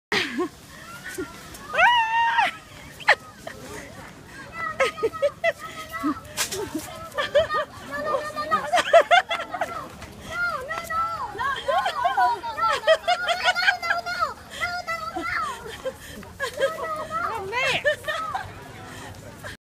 DEER IN NARA
They know when you have bought the deer crackers. They followed me around fast.